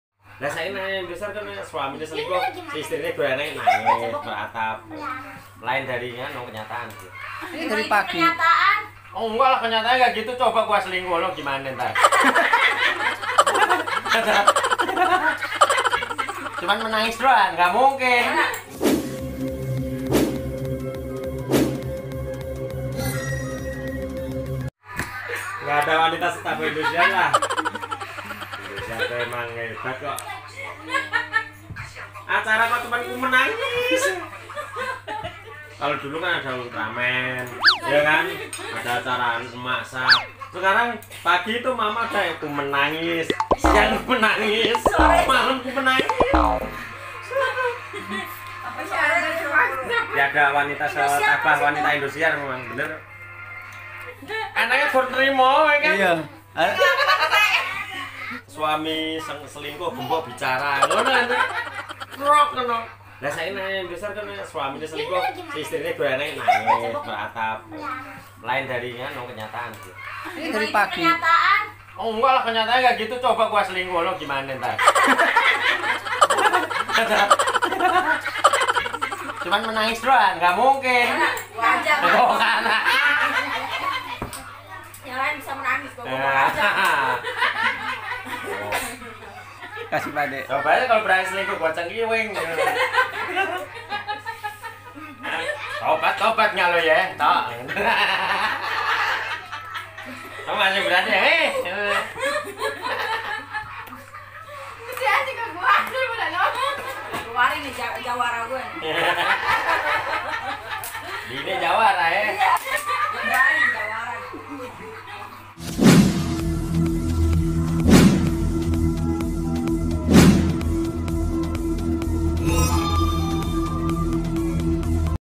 Tanggapan tentang Film Indosiar
ini tanggapan kawan sya soal film indosiar